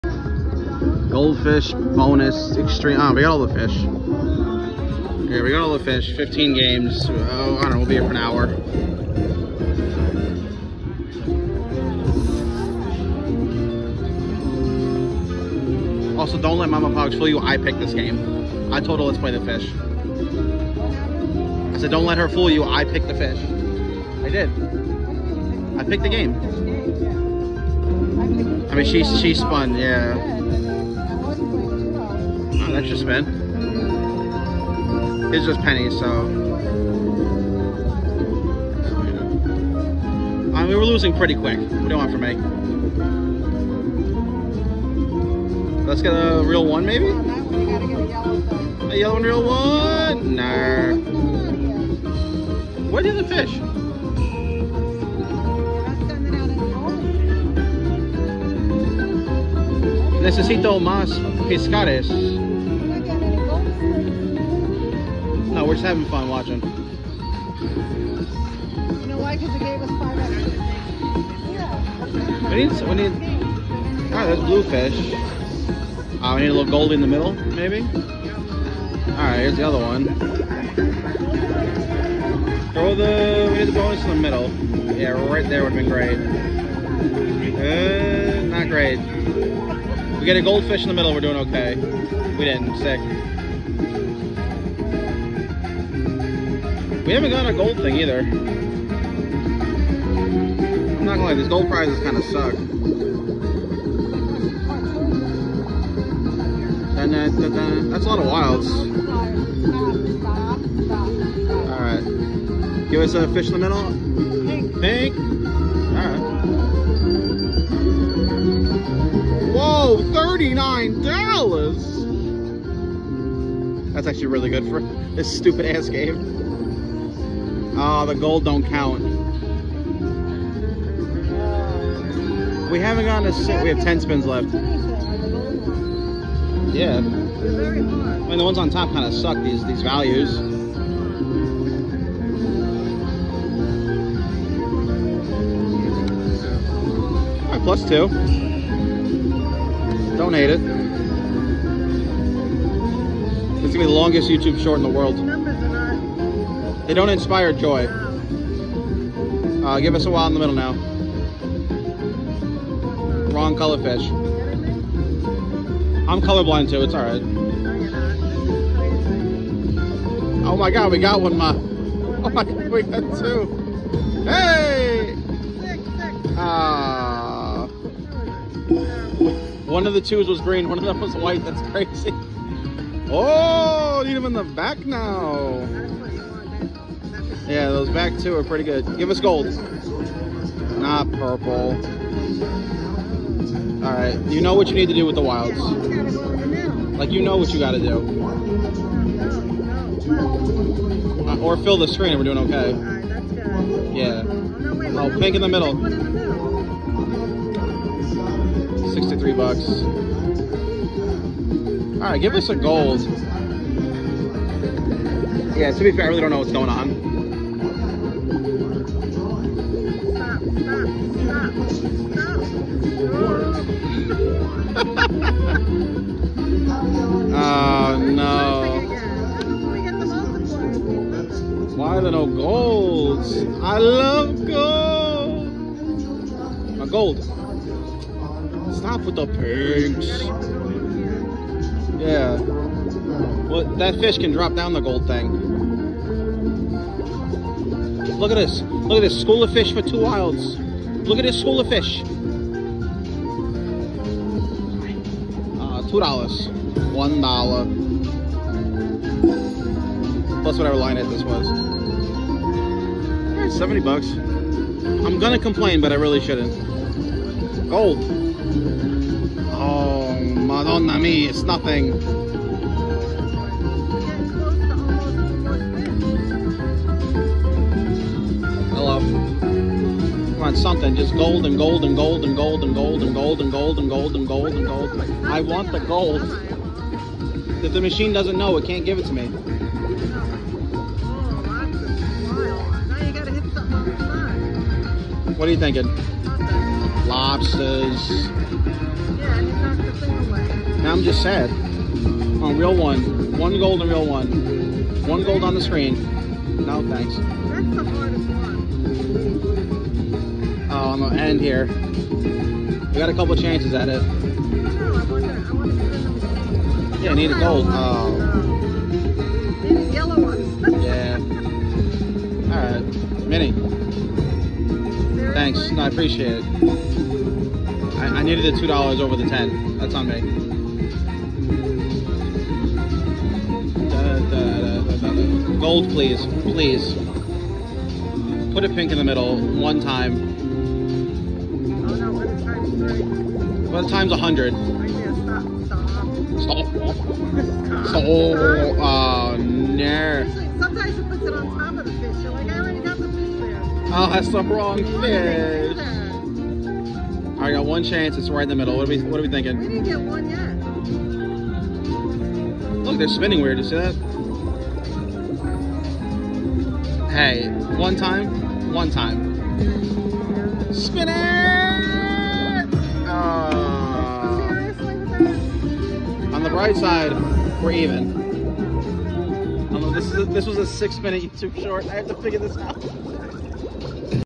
Goldfish Feeding Time ALL 3 FISH!
goldfish feeding time triple pop. that's it.